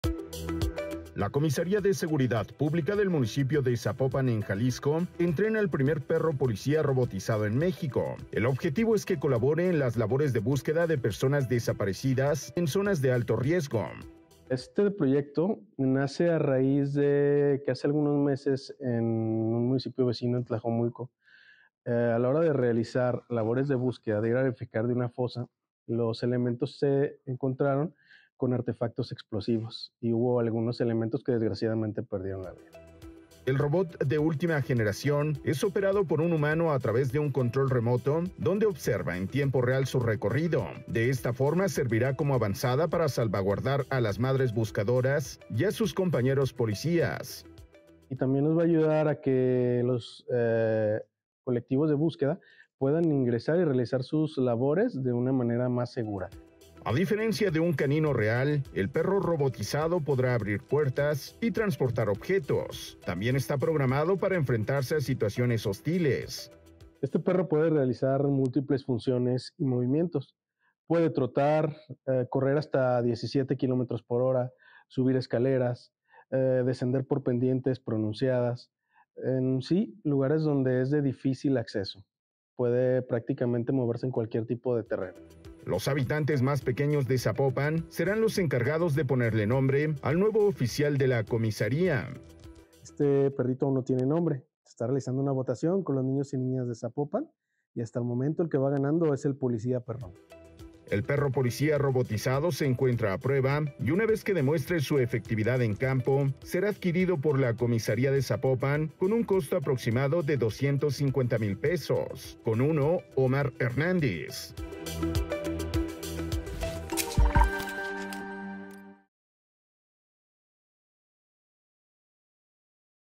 Entrenan en Zapopan Jalisco al primer perro policía robotizado
La comisaría de seguridad pública del municipio de Zapopan en Jalisco entrena al primer perro policía robotizado en México

El objetivo es que colabore en las labores de búsqueda de personas desaparecidas en zonas de alto riesgo.